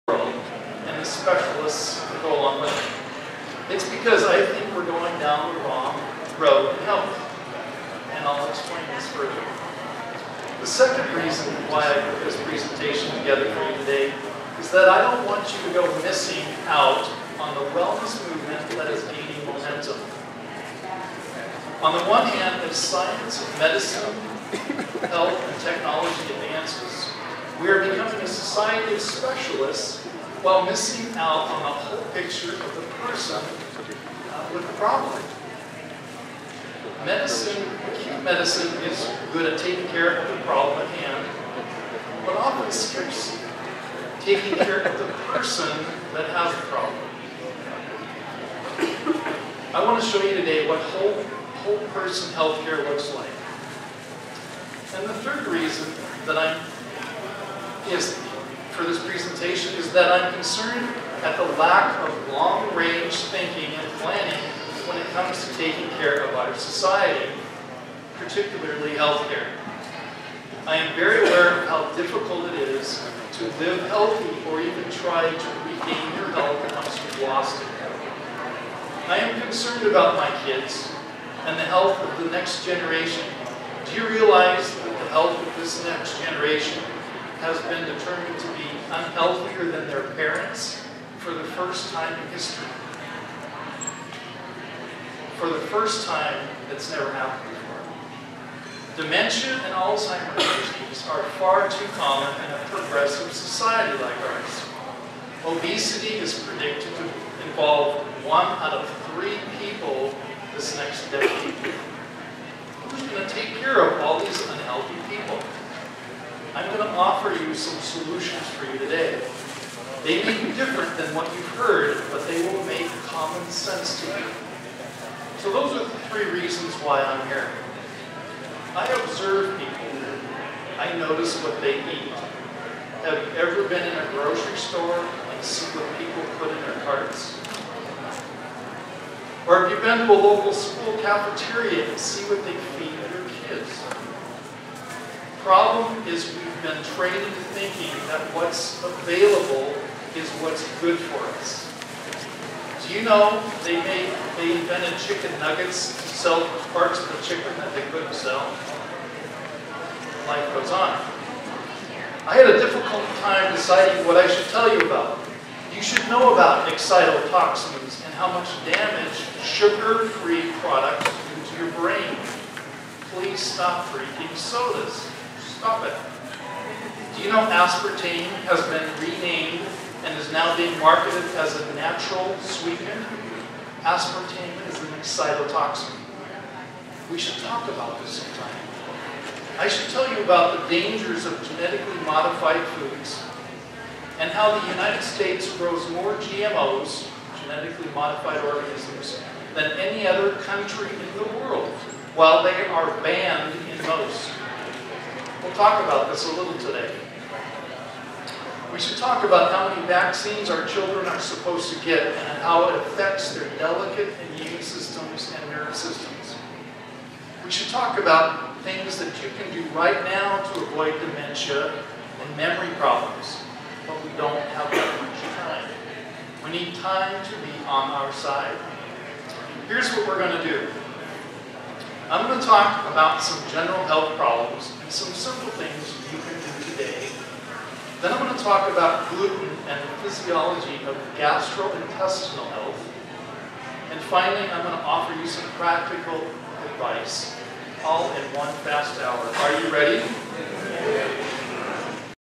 Gluten free, gastroenteritis, digestive tract
Gluten free, gastroenteritis, digestive tract: a public presentation by Dr. Peter Lind